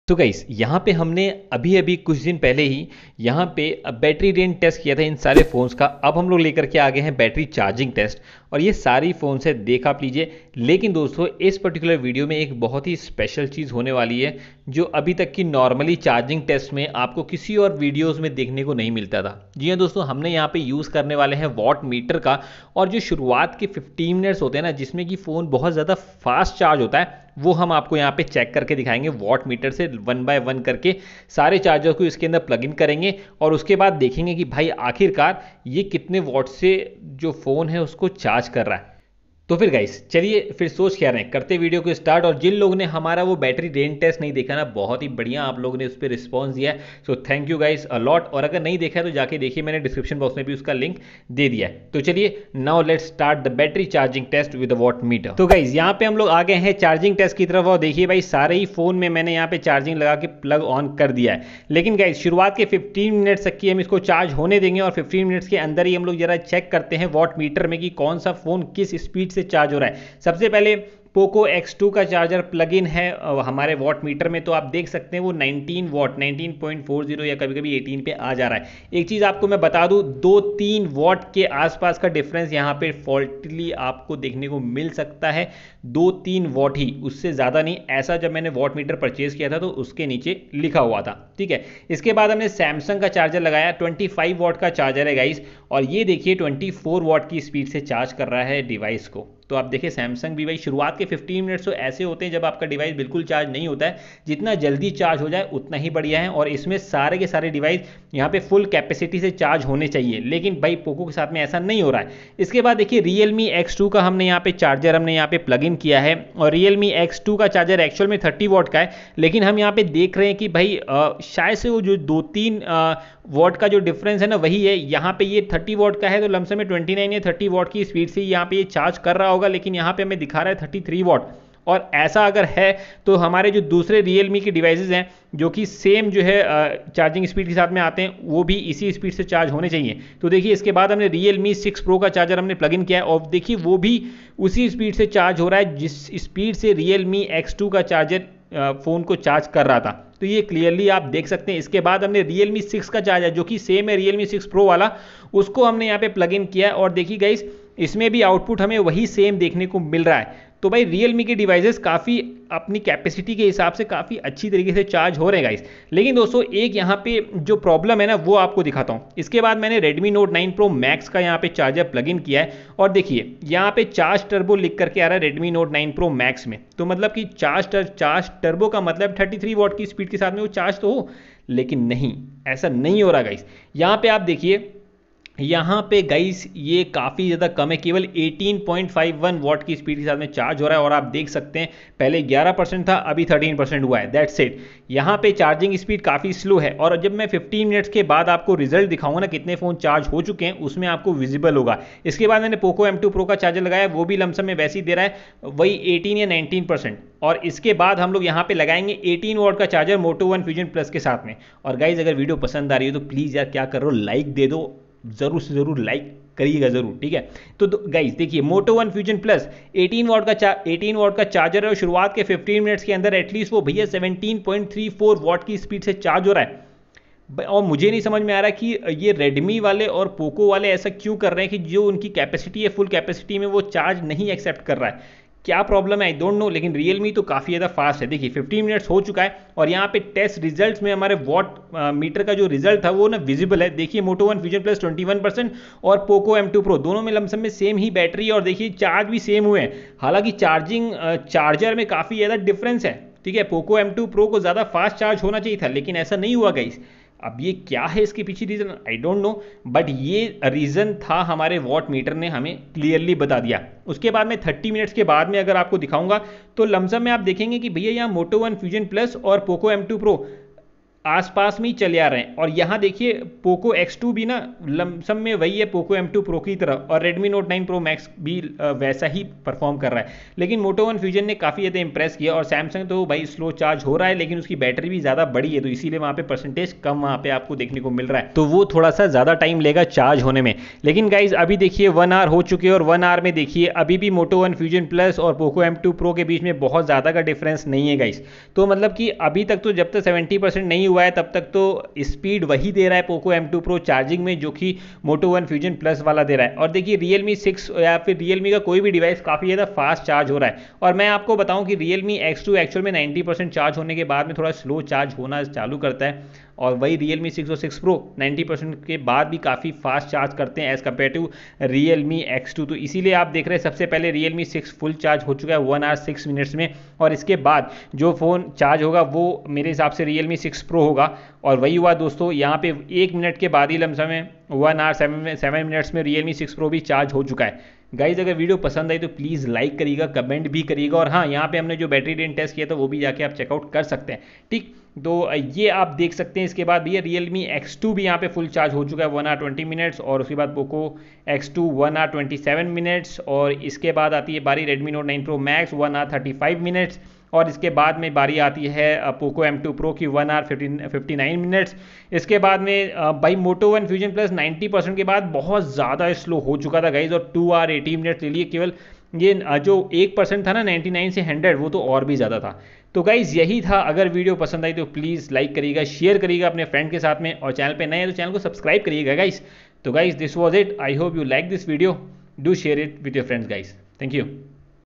Samsung M31s vs Poco X2 vs Realme X2 vs Realme 6 Pro vs Redmi Note 9 Pro Max vs Poco M2 pro
Samsung M31s vs Poco X2 vs Realme X2 vs Realme 6 Pro vs Redmi Note 9 Pro Max vs Poco M2 pro vs realme 6 vs Motorola one fusion plus batter Test with Watt Meter
In our earlier video I have already shown battery drain test among all these devices, and now in this video I shall be showing you the battery charging test with Watt Meter in first 15 Min. Let's find which phone is the fastest and soon we shall be ordering realme 7 and realme 7 Pro so you shall get to see that phone included in these type of test soon and yes, poco x3 is launching today, we shall buy that too for you all.